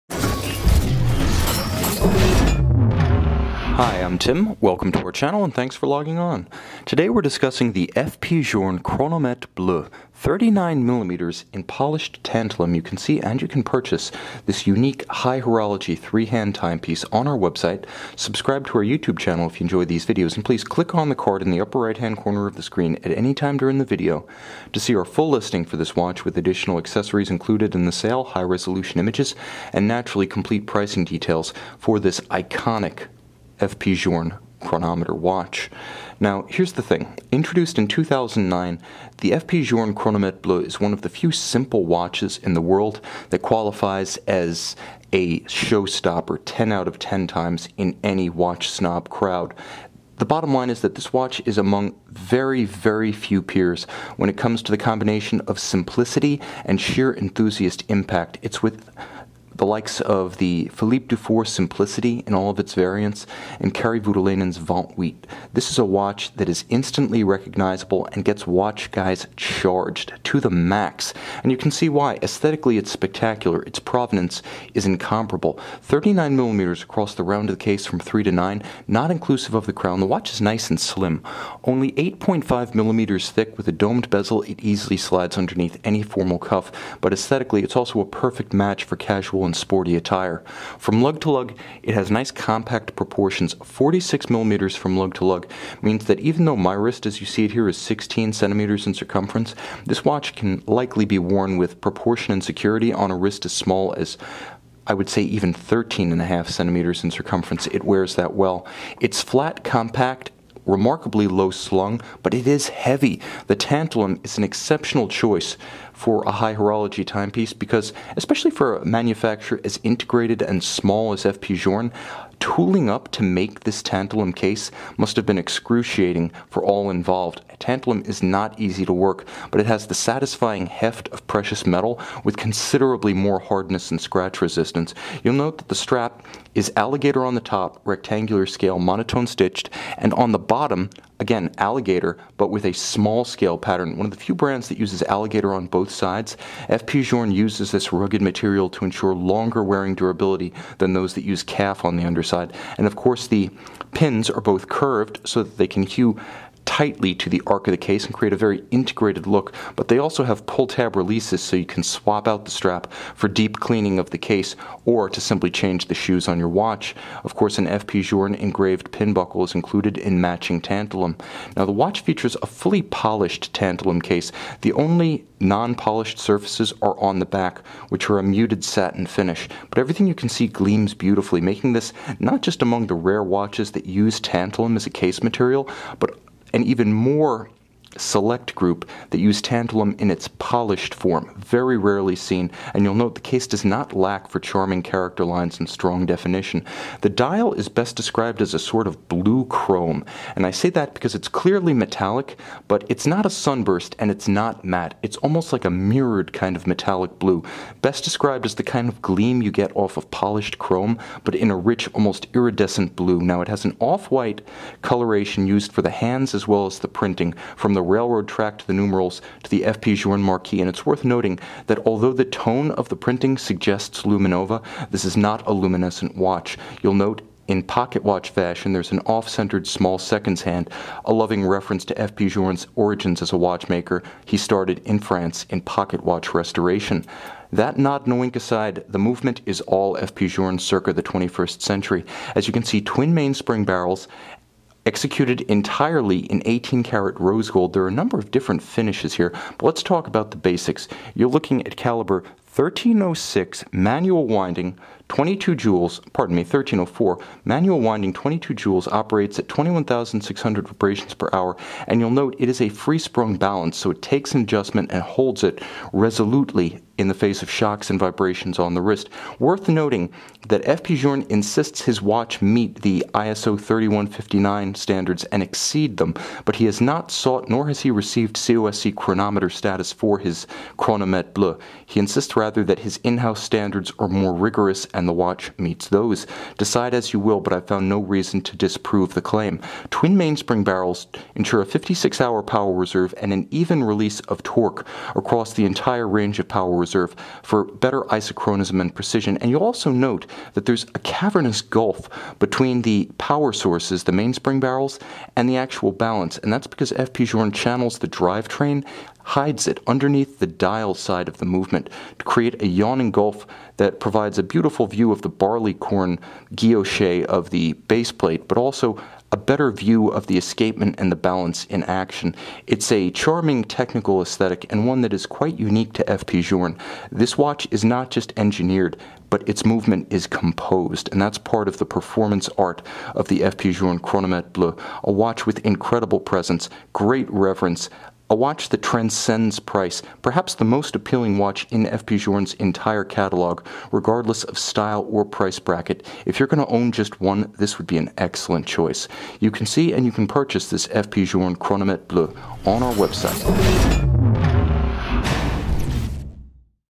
F.P. Journe Chronometre Bleu Luxury Watch Review
F.P. Journe Chronometre Bleu features a 39mm tantalum case and a finely adorned crown on its right flank. The dial is chrome blue with cream-colored Arabic numerals and minutes chapter ring. The small seconds subregister at 7:30 exhibits a fine concentric circle guilloche that adds depth to the already enticing dial. The sapphire caseback exhibits the main event of this Chronometre Bleu: the intricately decorated Calibre 1304 movement produced entirely of 18K gold. This F.P. Journe Chronometre Bleu is on a blue alligator strap with tantalum pin buckle